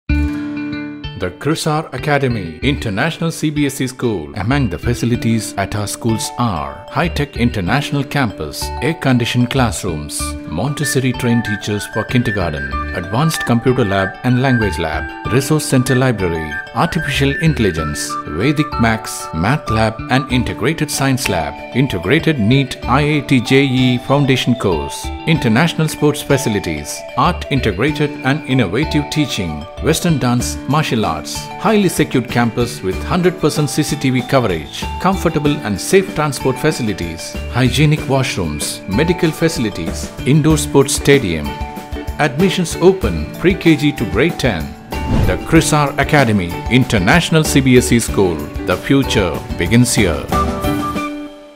Admissions Open 2022-23 | From Pre-Kg to Grade 10 | The Krisar Academy
The Krisar Academy aspires to impart knowledge and values to students and nurture their talent, enthusiasm, and creativity to pursue excellence through high-quality education. It was founded in the year 2018 with the objective of offering holistic education to the children in the villages around the major towns, such as Arani, Timiri, Arcot, Vellore, Ranipet, and Walajapet, and eventually becoming the best CBSE school in Arani. The school believes that education is the most powerful weapon that can be used to transform the world. The school strives to provide the children with both a scholastic and a co-scholastic learning experience.